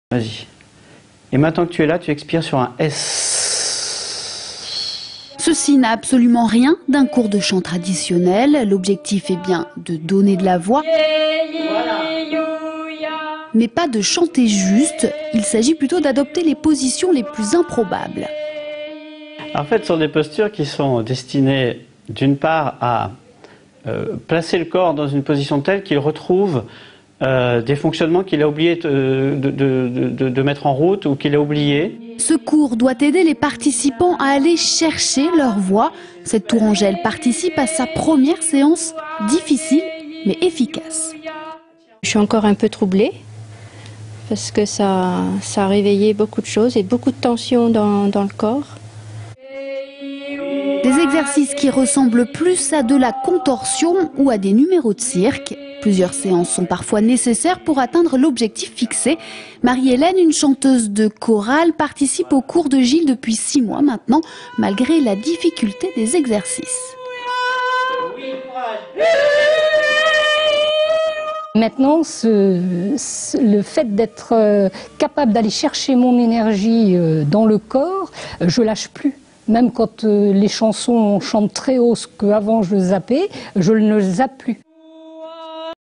Tours : Le chant adoucit aussi les moeurs ?
Tours : Le chant adoucit aussi les moeurs ? - Tours - Des cours de chants thérapeutiques, c'est ce que propose l'université du possible à Tours sous forme de stage ou de cours collectif. Il s'agit bien de chanter mais pas forcément juste. Les exercices restent très physiques, il faut aller chercher sa voix le plus loin possible. Objectif?